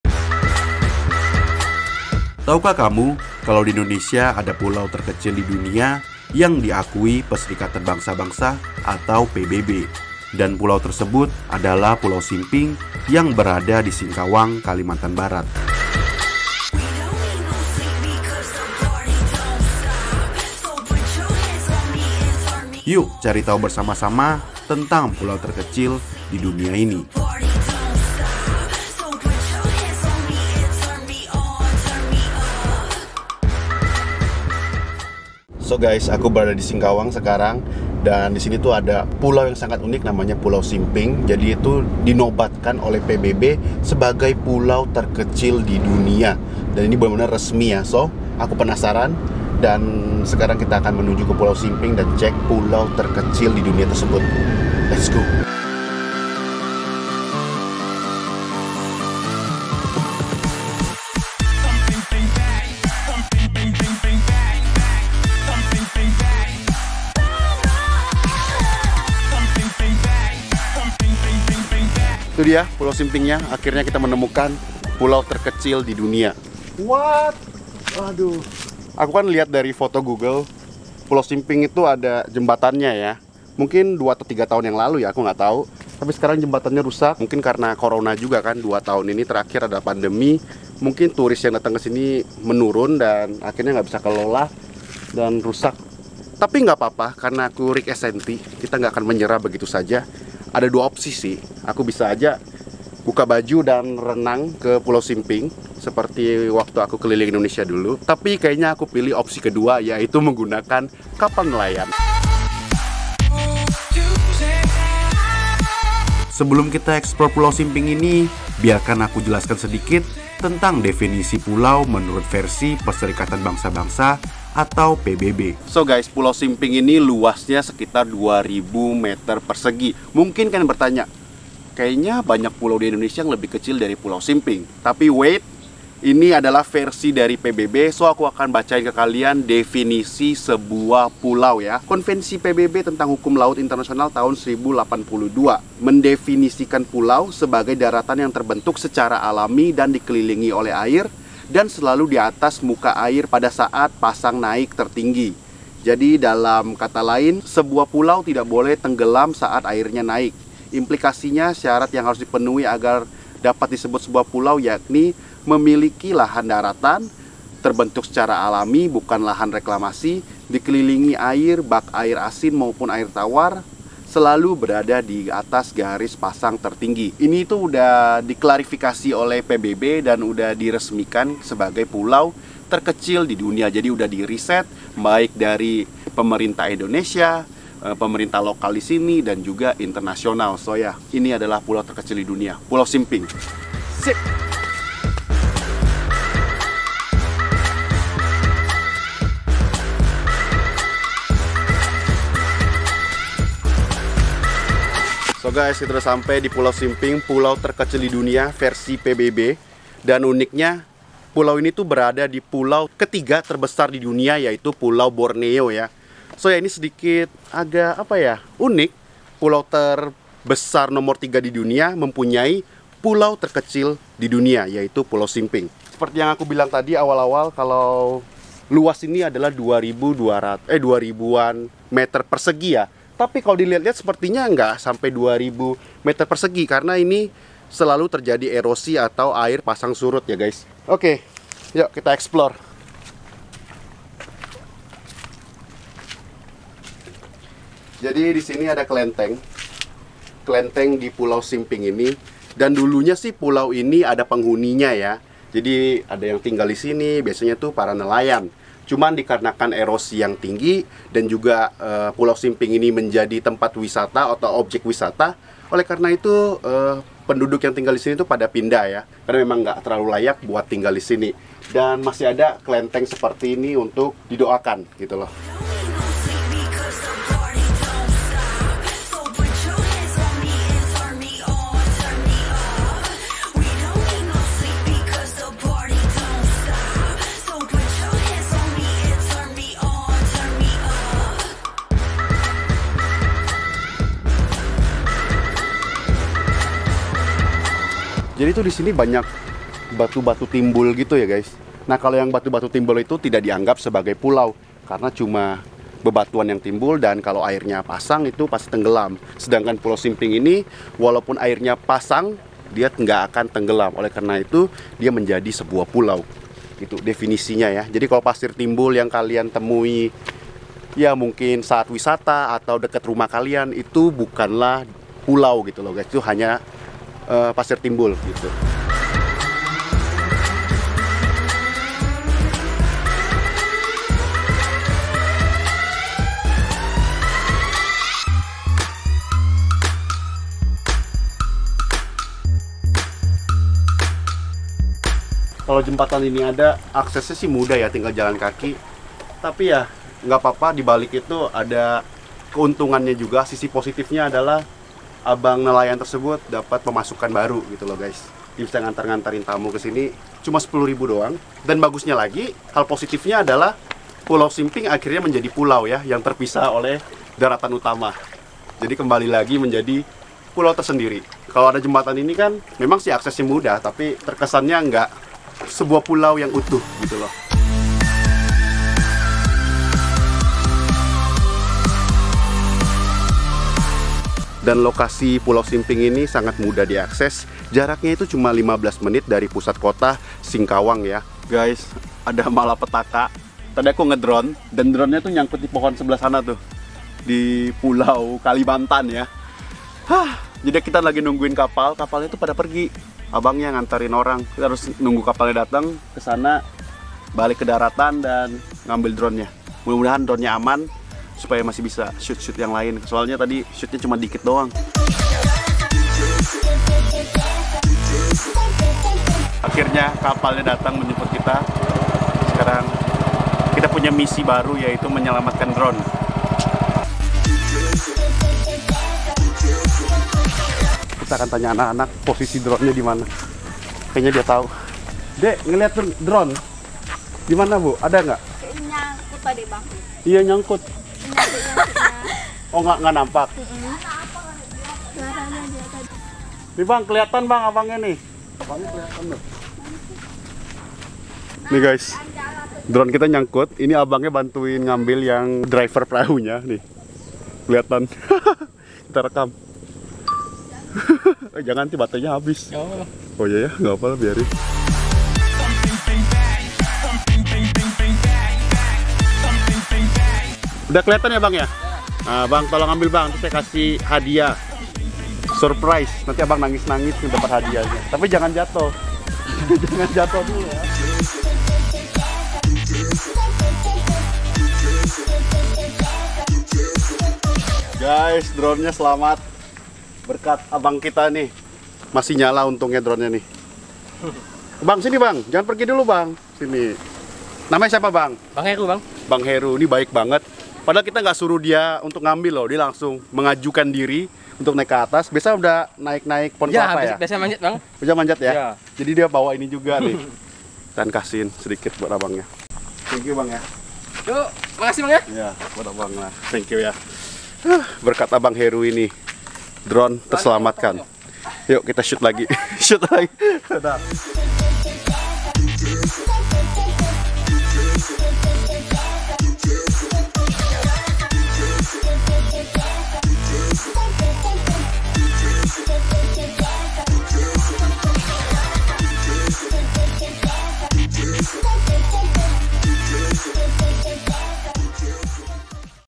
TERNYATA PULAU TERKECIL DI DUNIA ADA DI INDONESIA! DIAKUI PBB "PULAU SIMPING"
Tahukah kamu? kalau di indonesia ada pulau terkecil di dunia yaitu pulau simping yang berada di singkawang, kalimantan barat.

Support by: LUXURY TRANSPORT BALI
alamat: jl.puri mumbul permai jimbaran, kab. badung, bali.